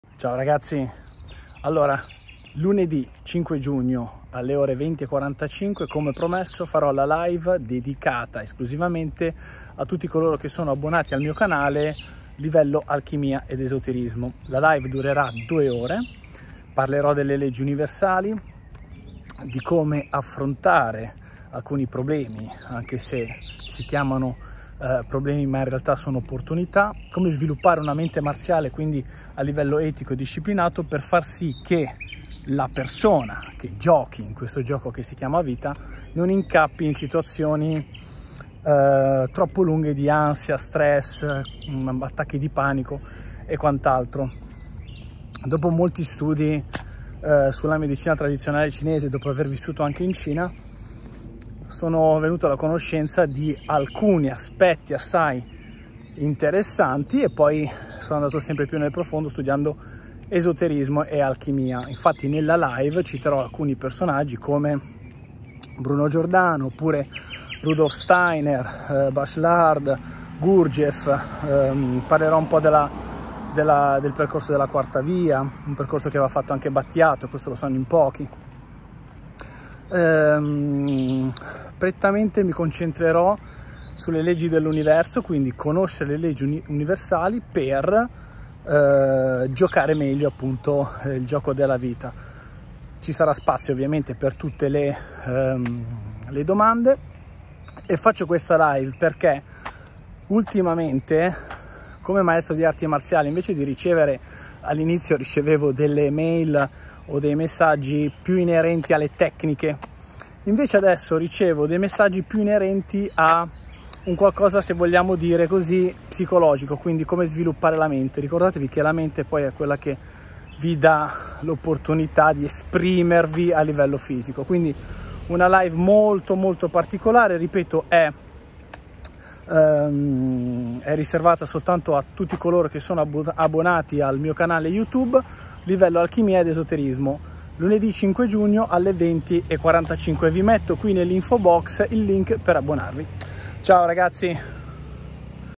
Vi aspetto alla LIVE riservata agli abbonati | Alchimia ed Esoterismo
Lunedì 5 Giugno ore 20:45
Nasciamo in questo mondo e viviamo immersi in alcune regole e leggi universali.
Anticamente alcuni personaggi che vennero iniziati ad un percorso di crescita esoterico e alchemico conoscevano queste leggi, ma al giorno d'oggi poche sono le persone che realmente conoscono le leggi universali e quindi le regole del gioco i grandi personaggi come Rudolf Steiner e altri ci hanno aiutato nella comprensione e nella consapevolezza dell'invisibile.
Dopo molti anni nello studio delle arti marziali e dopo aver incontrato grandi maestri, molto tempo fa ho iniziato a seguire un percorso esoterico coadiuvato con la medicina tradizionale cinese e l'alchimia e così mi sono addentrato in quello che l'essere umano scoprendo che il vero segreto per un vero maestro di arti marziali è la comprensione dell'essere e la conoscenza.
La vera differenza la fa la conoscenza, come diceva il grande Salomone se proprio devi cercare qualcosa cerca la conoscenza.
In questa live della durata di due ore affronteremo quello che è  l'essere umano e la sua comprensione, attraverso i corpi che lo abitano, la sua mente e le sue emozioni tutto nel rispetto delle leggi universali.